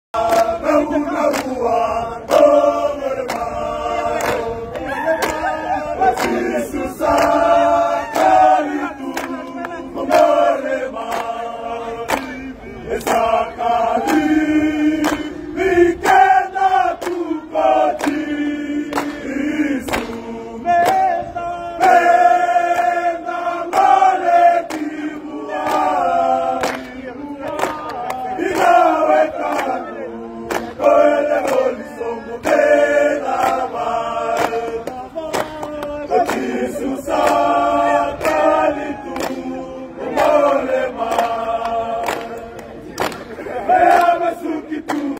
North Sydney Bears & the Kaiviti Silktails
An amazing moment after the Bears win on 23rd May at North Sydney Oval when the Kaiviti Silktails Rugby League Football Club team mates celebrated Penioni Tagituimua's debut for the mighty Red & Black.